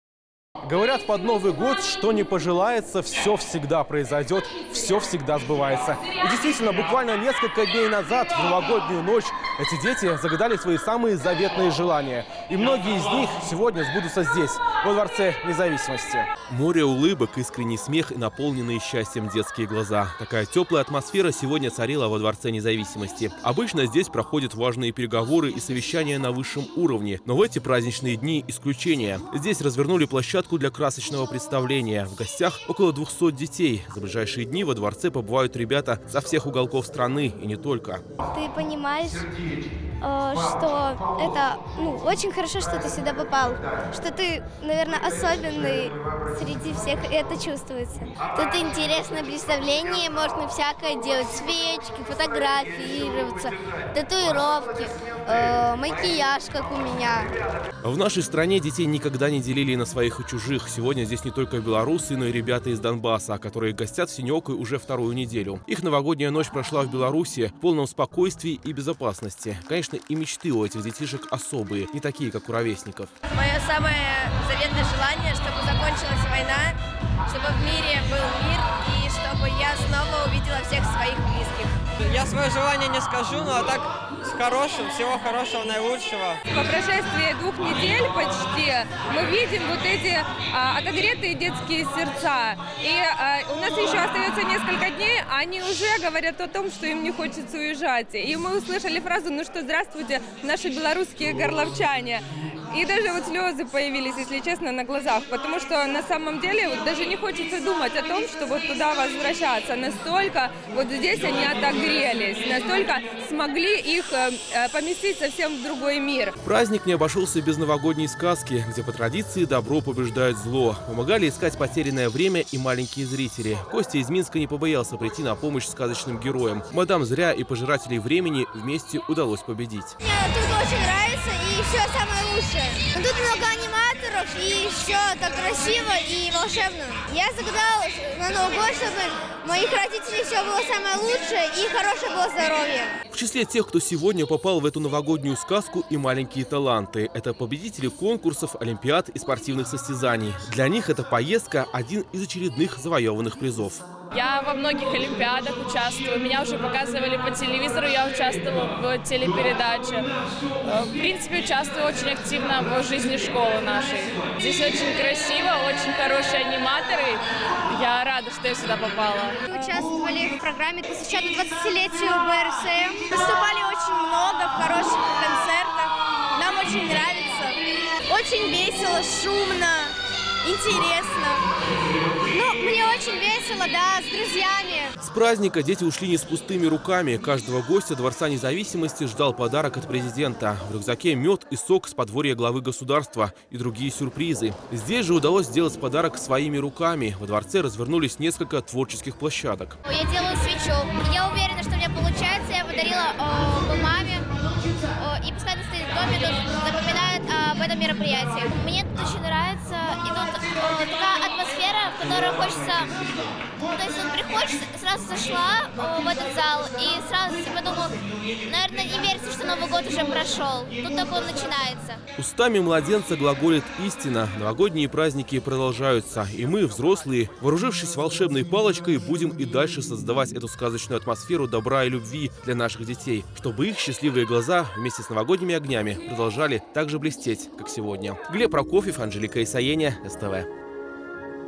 Новогодний утренник во Дворце Независимости! Что Президент подарил деткам? / Новый год
У белорусских школьников каникулы и настоящее время исполнения желаний. 4 января во Дворце Независимости стартовала серия новогодних утренников. На красочное представление пригласили всех тех, кто в эти праздничные дни особенно ждет чудес и подарков. Какие сюрпризы ждали маленьких гостей? / Репортаж.